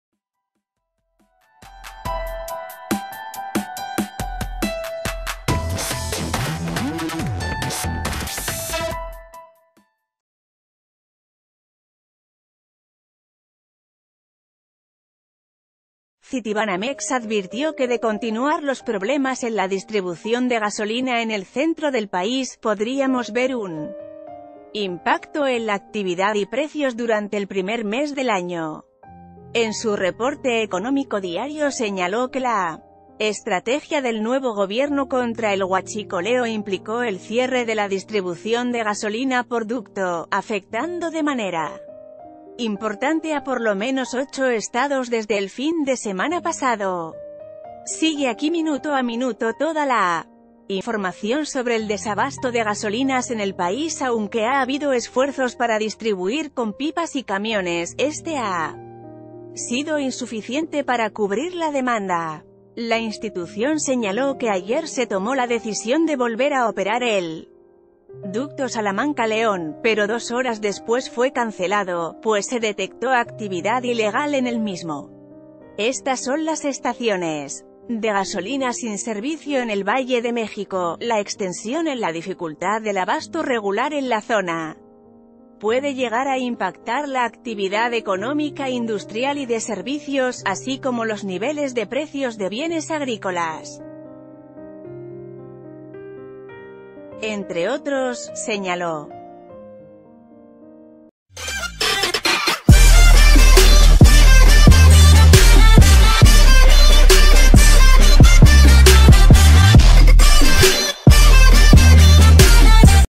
Advierte Citibanamex de impacto en precios si continúa la falta de gasolina
Citibanamex advirtió que de continuar los problemas en la distribución de gasolina en el centro del país, podríamos ver un impacto en la actividad y precios durante el primer mes del año. En su reporte económico diario señaló que la estrategia del nuevo gobierno contra el huachicoleo implicó el cier...